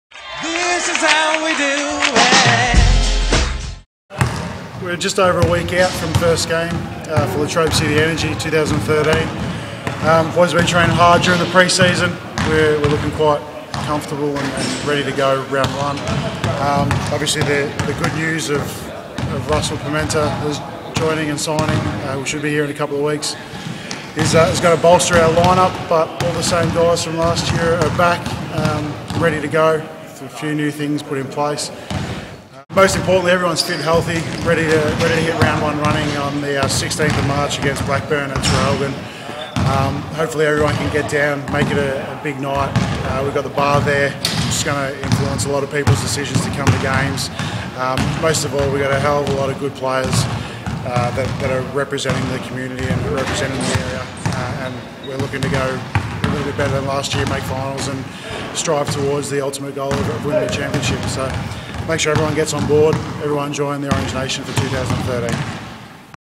Energy Basketball - Les Stothard
Latrobe City Energy Division 1 Men Head Coach Les Stothard talks about the season ahead; encouraging everyone to join the Orange Nation in 2013.